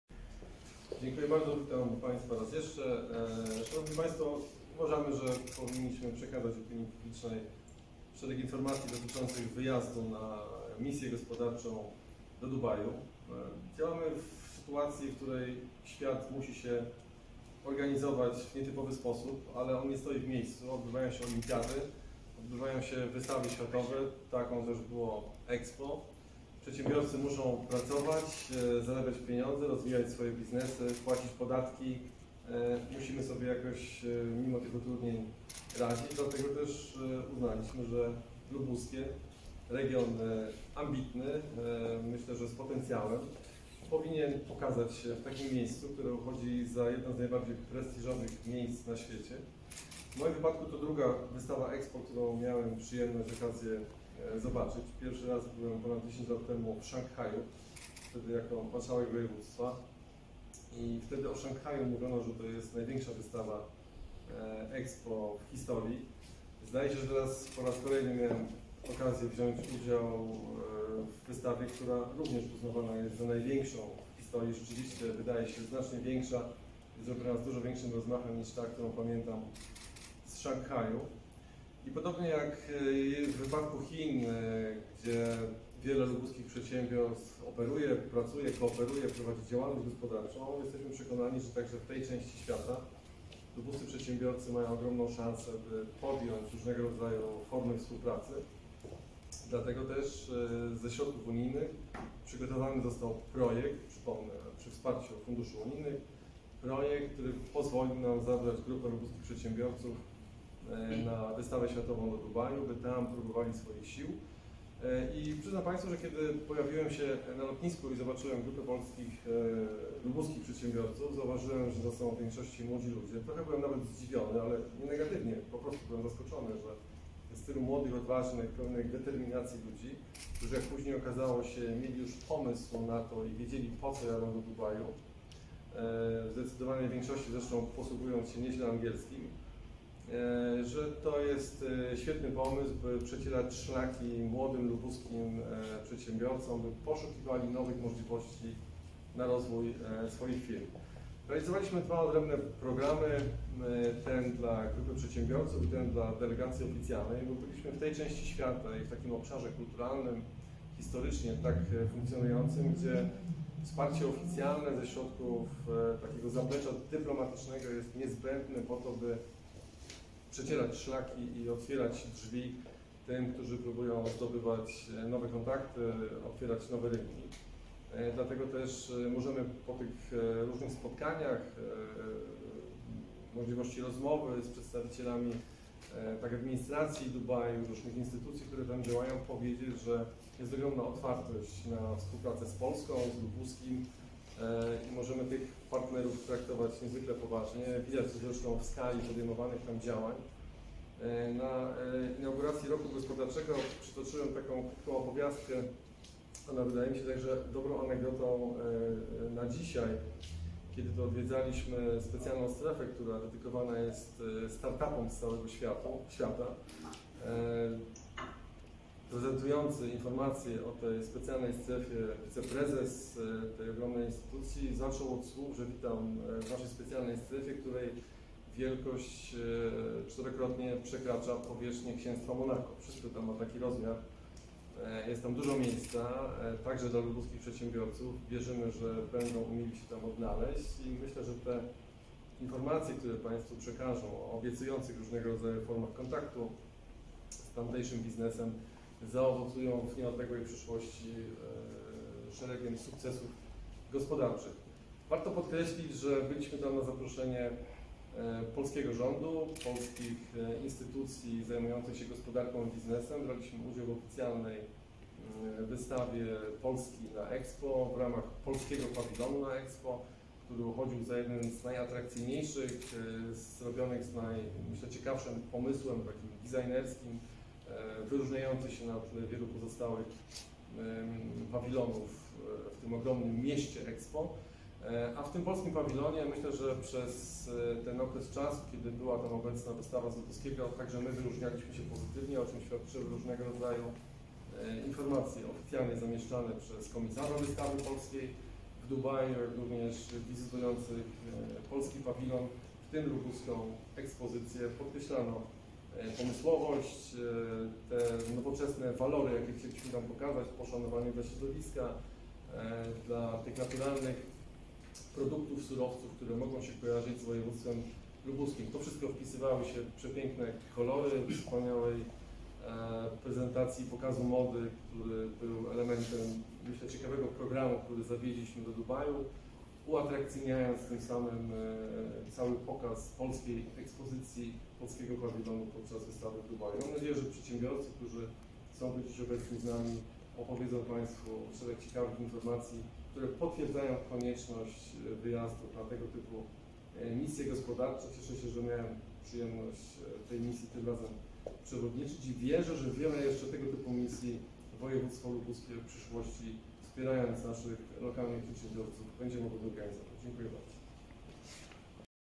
Marcin Jabłoński o wizycie w Dubaju - konferencja prasowa 10.02.2022 r.
Fragment konferencji prasowej z wypowiedzią członka zarządu województwa Marcina Jabłońskiego na temat misji gospodarczej w Dubaju. Konferencja odbyła się w dniu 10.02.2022 r.